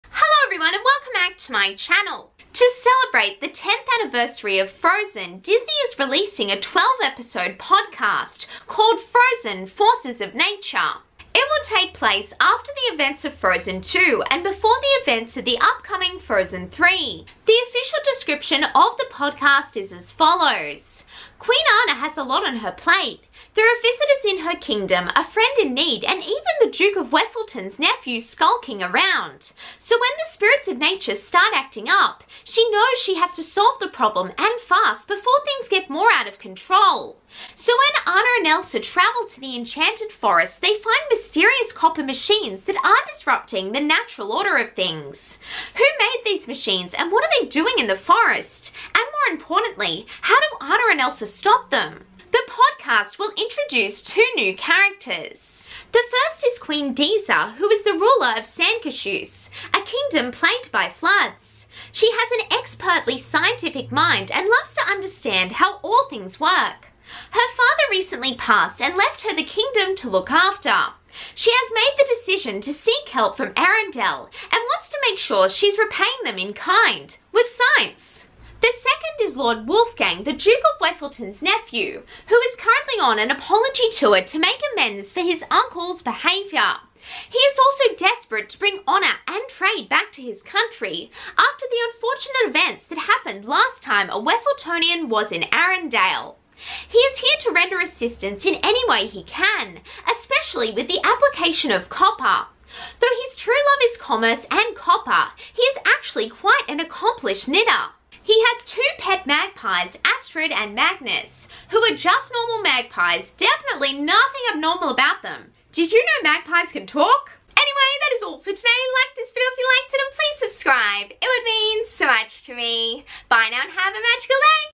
Frozen: Forces of Nature Podcast | Plot details and new characters | Queen Disa and Lord Wolfgang
Recently Disney announced a 12 episode podcast series called Frozen: Forces of Nature.  In this video I discuss plot details and the new characters (Queen Disa, Lord Wolfgang, Astrid and Magnus).  Watch for all the details!